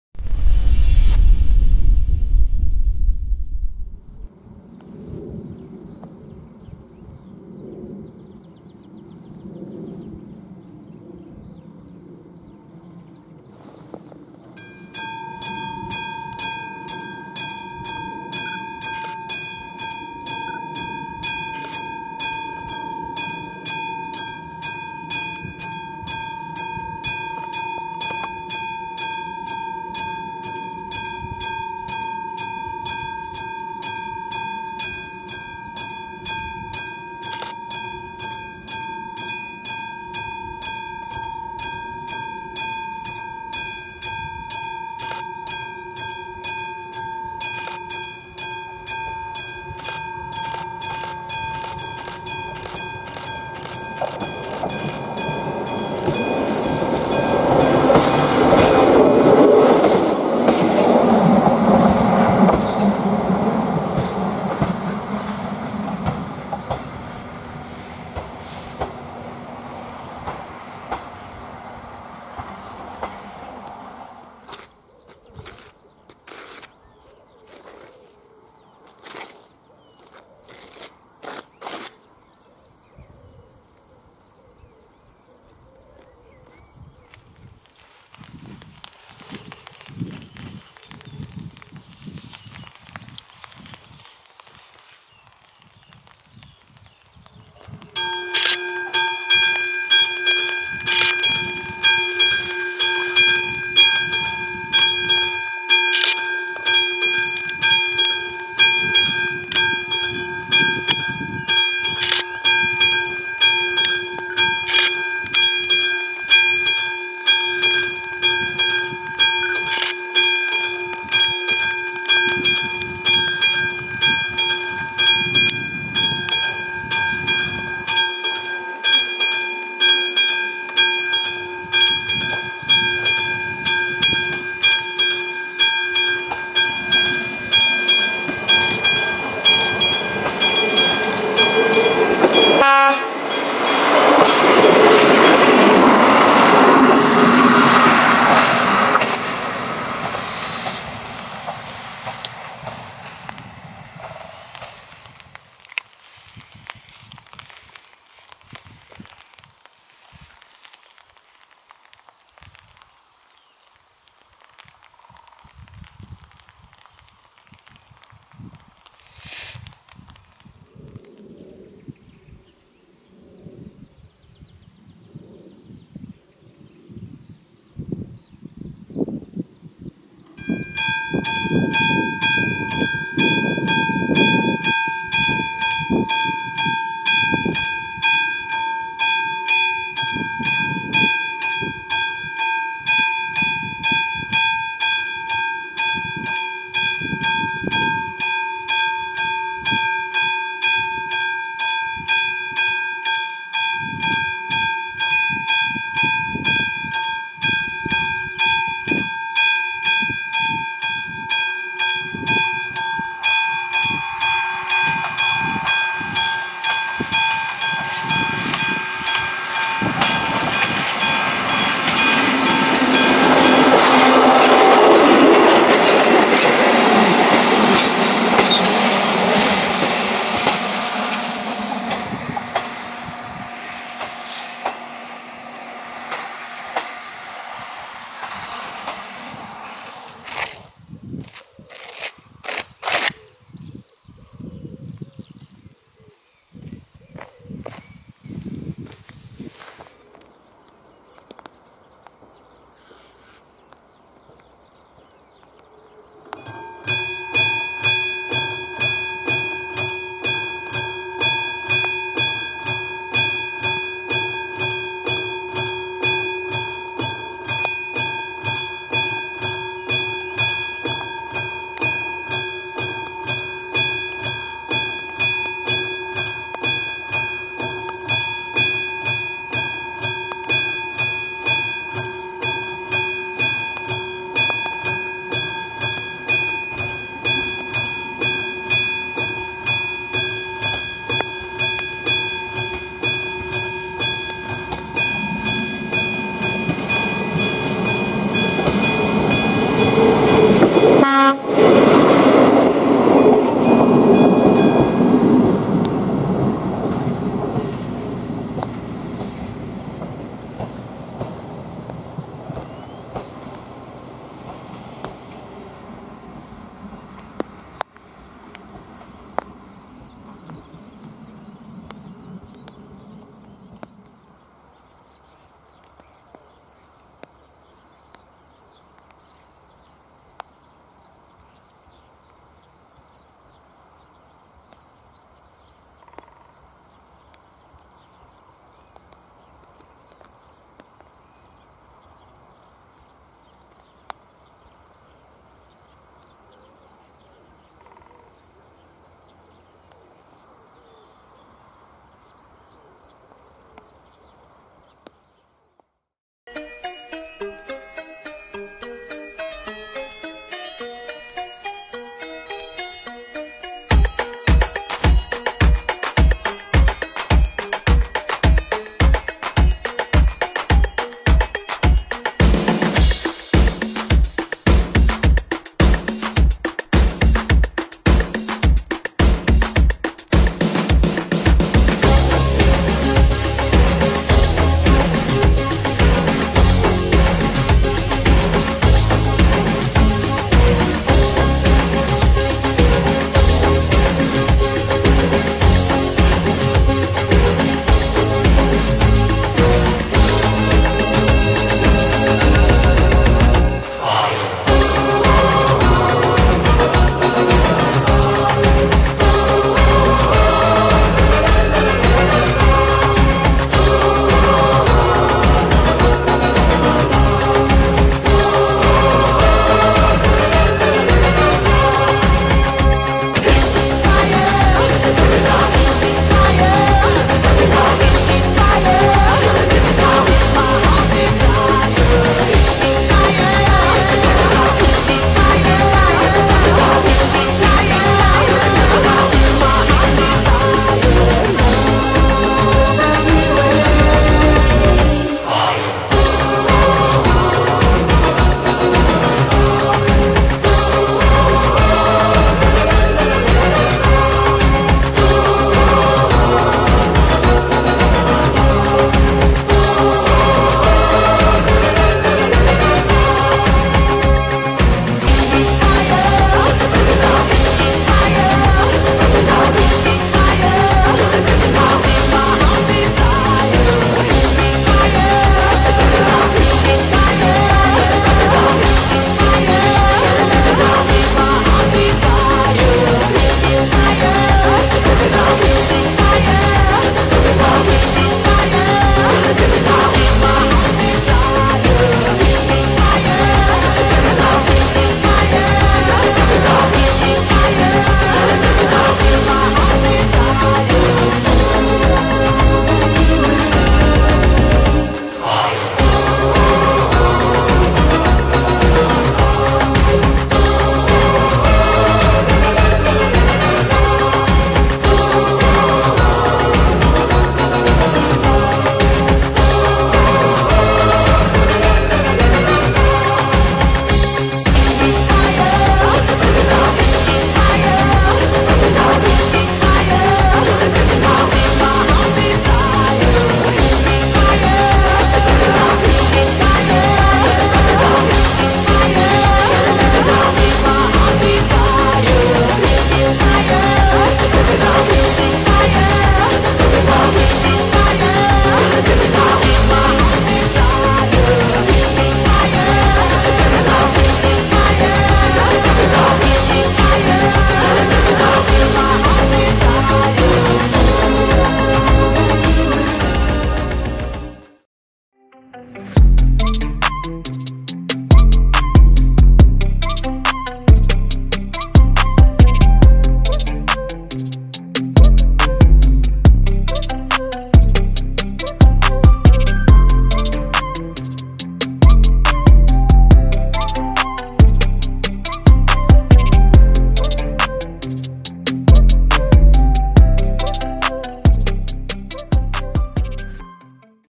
Železniční přejezd Zalešany (08.04.2023)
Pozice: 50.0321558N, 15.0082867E
Trať: (012) Pečky - Bečváry
Číslo přejezdu: P4970
Výstražníky: AŽD71
Spolupráce: xxx